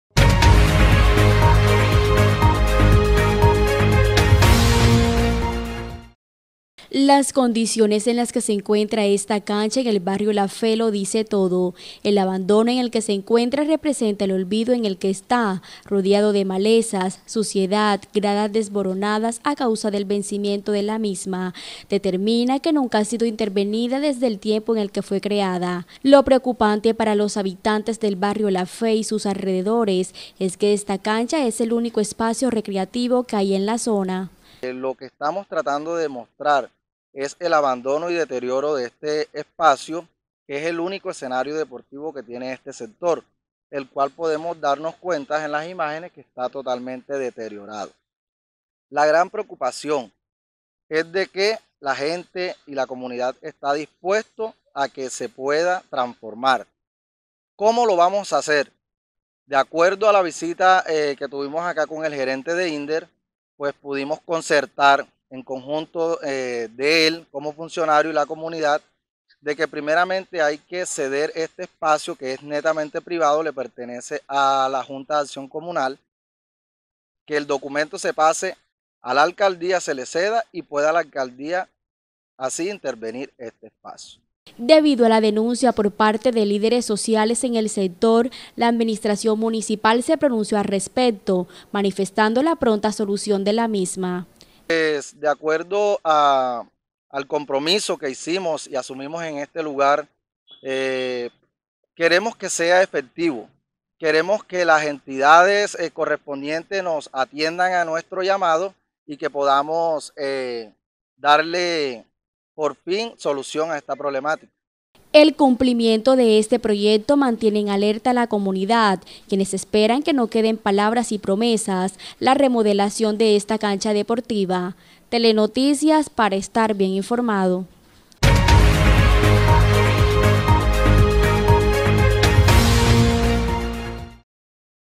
CANCHA EN COMPLETO DETERIORO.
En TelenoticiasEnTuBarrio La cancha del barrio La Fe de Sincelejo se encuentra en completo abandono, las comunidades de este sector le piden a la administración local cumplir con el pacto acordado.